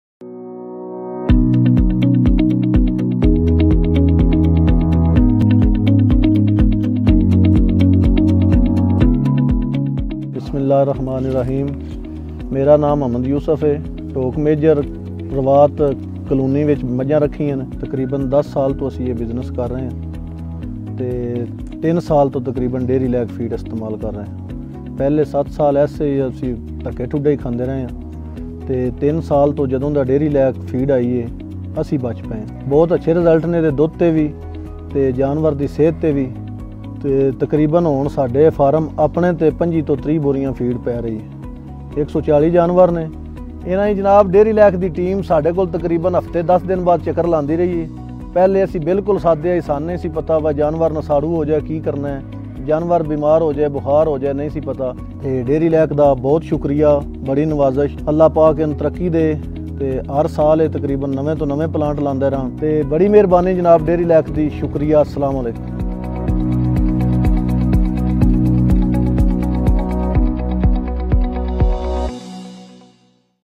Dairy lac Pakistan Muhammad Yousuf#DairyLac
Muhammad Yousuf is our farmer from Rawat Islamabad. He is doing dairy farming since last 10 years. He is DairyLac feed user since three years. He is having a healthy herd and getting good milk yield from them. Let's listen his story.